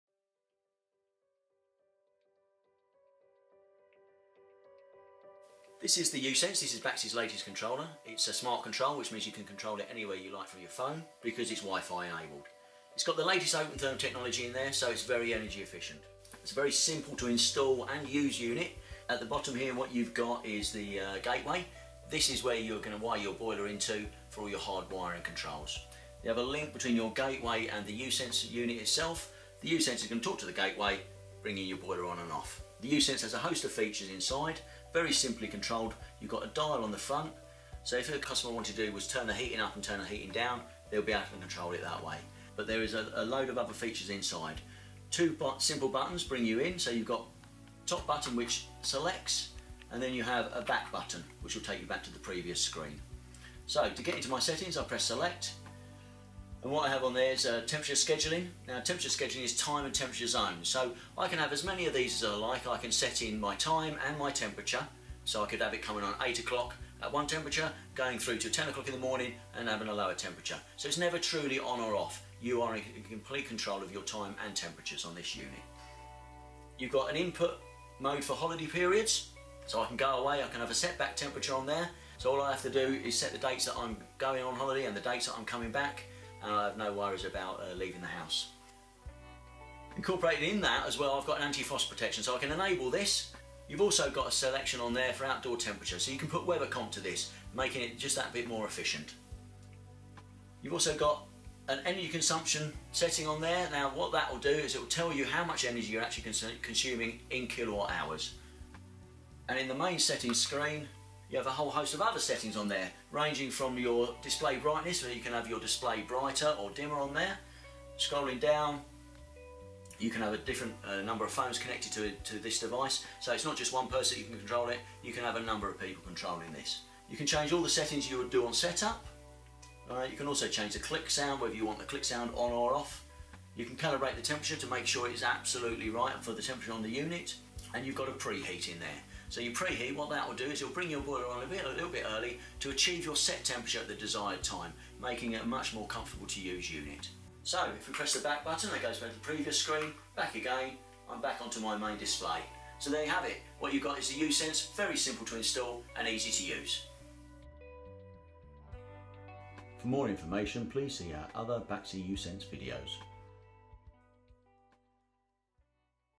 Baxi uSense smart room thermostat - Overview | central heating controls | smart thermostats
Overview of the Baxi uSense smart room thermostat:

The Baxi uSense is compatible with all combi boilers using the 230V switch live. Additional features, such as boiler fault diagnosis, are enabled when connected via OpenTherm.

The following Baxi boilers are OpenTherm compatible:

Baxi 600
Baxi 400
Baxi 200
Baxi 100